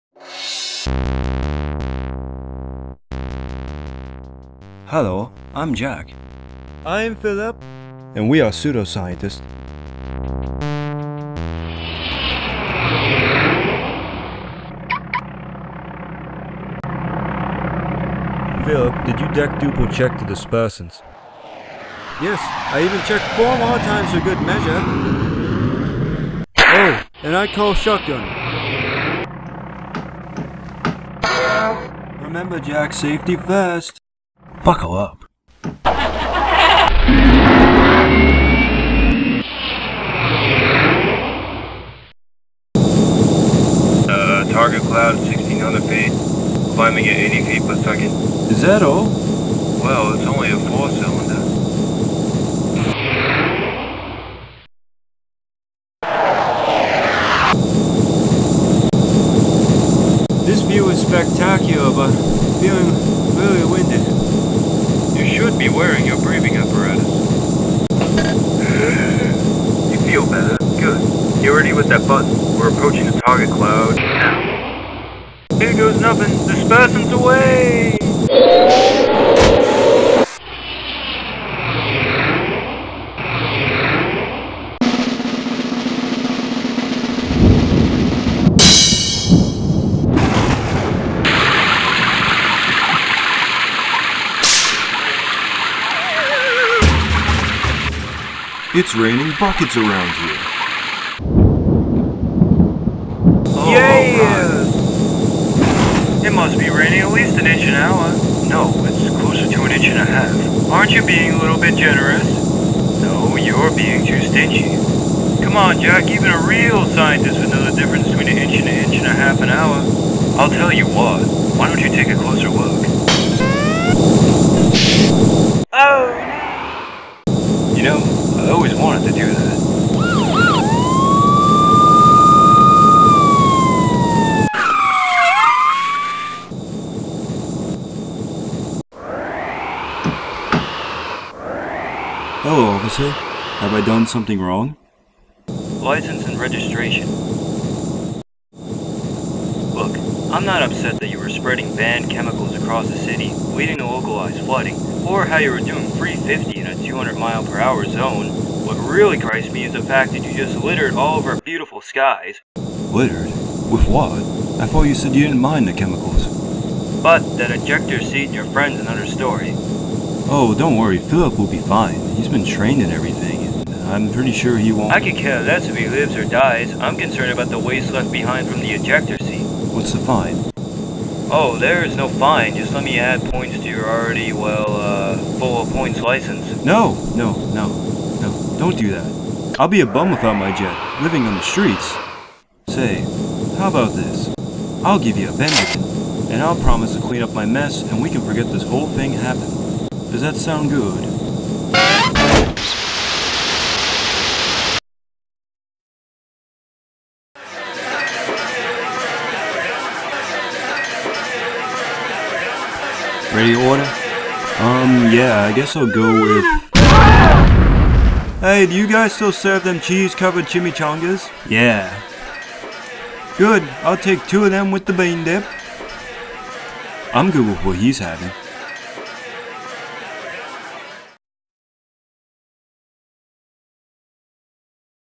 Sunshine Short : Cloud Seedin'
A cartoon short demonstrating cloud seeding I created.

(Animation specs: Artwork: MS-Paint 7. Animation: Frame-by-frame via Animation Shop 3. Editing: Powerdirector 10. Music: My own created with Anvil studio than remixed in Audacity.